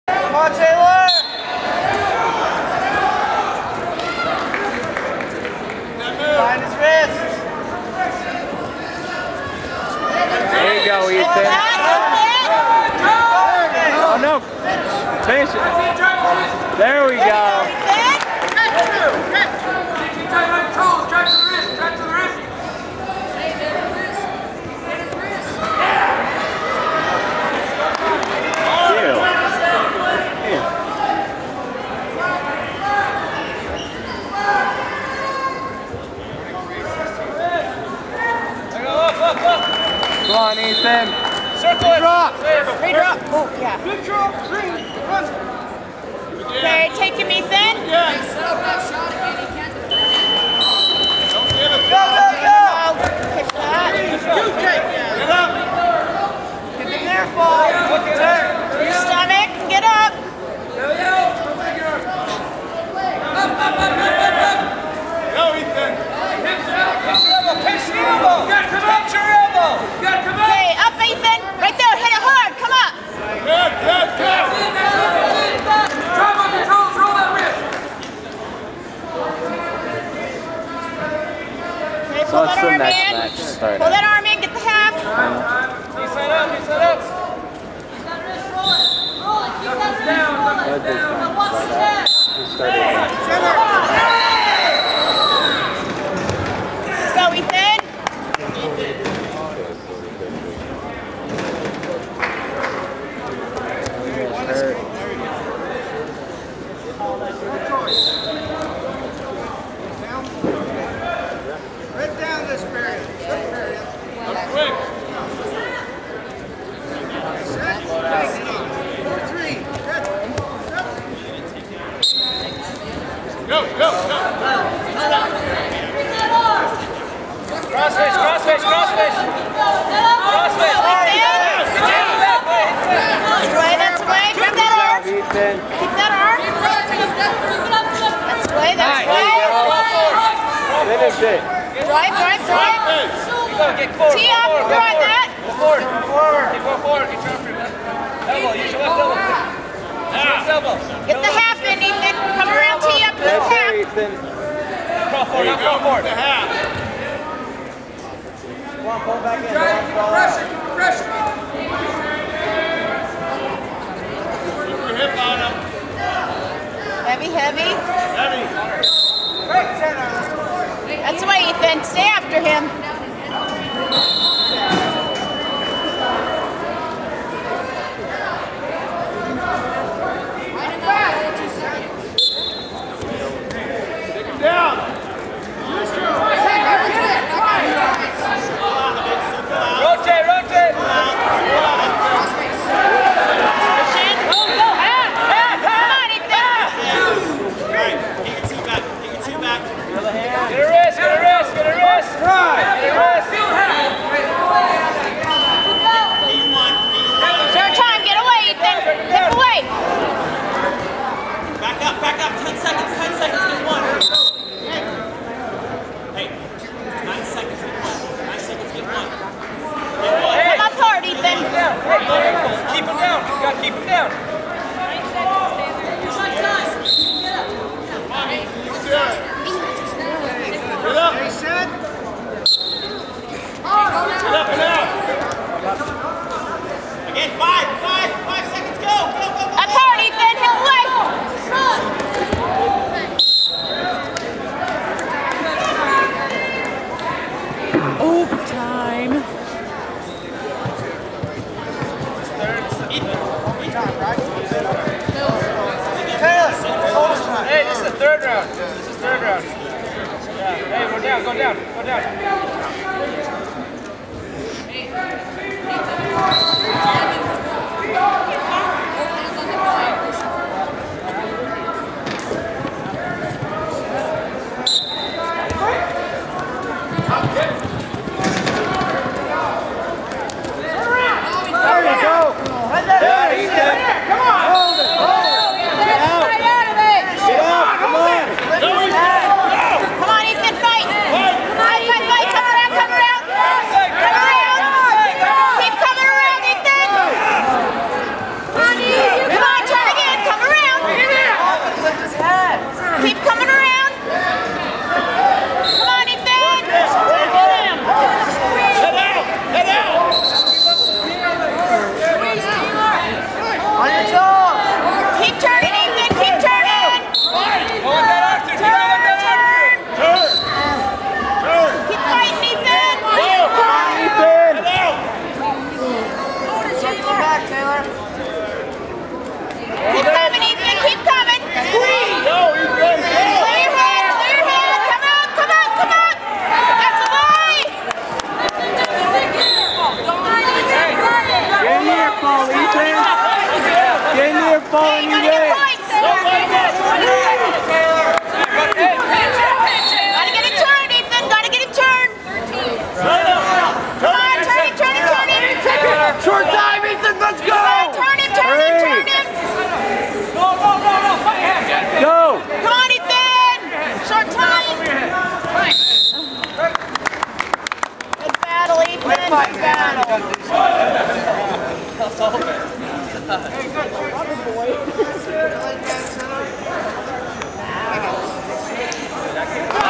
2013 Syracuse Scuffle |Taylor of Olympus vs Syracuse JV 120
These are high school wrestling matches from the 2013 Syracuse Scuffle held each year at Syracuse High School in Syracuse, Utah.  This dual was between Olympus High and Syracuse High School JV Team.